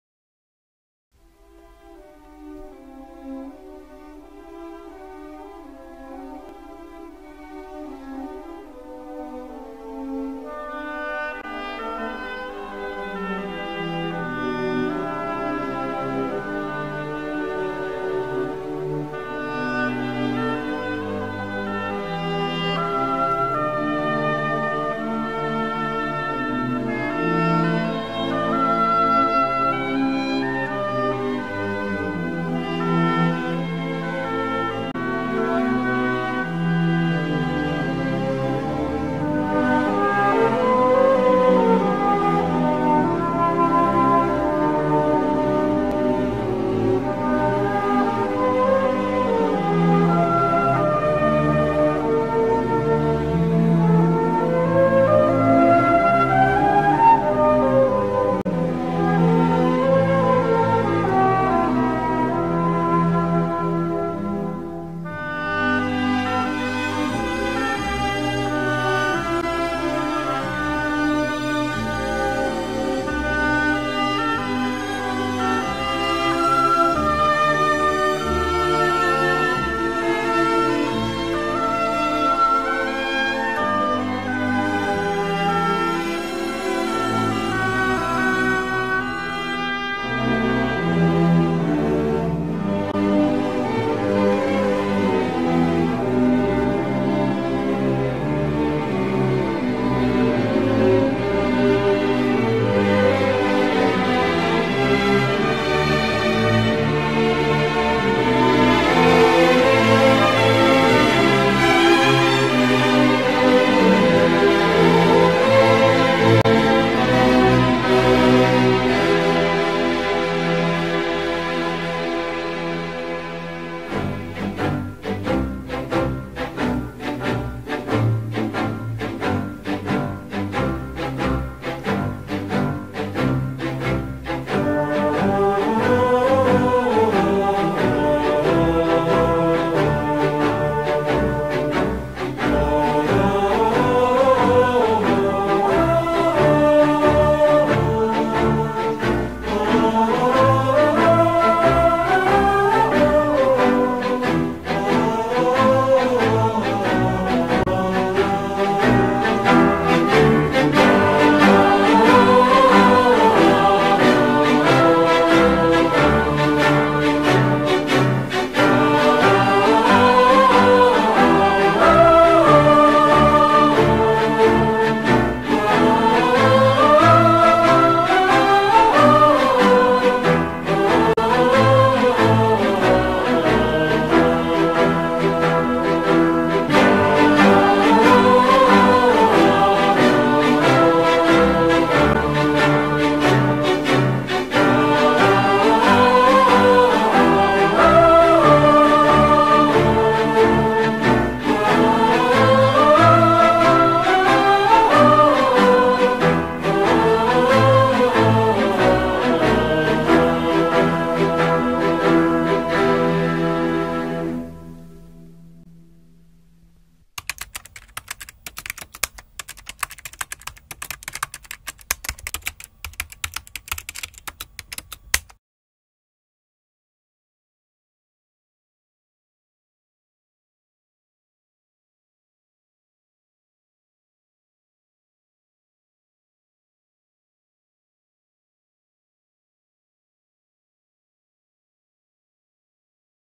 Vídeo 'La huella que perdura'
Per a l'acte de celebració del 40 aniversari de la històrica Assemblea de Barcelona de CCOO, fet el dilluns 11 de juliol de 2016 a la tarda a l'antiga Fabra i Coats de Barcelona.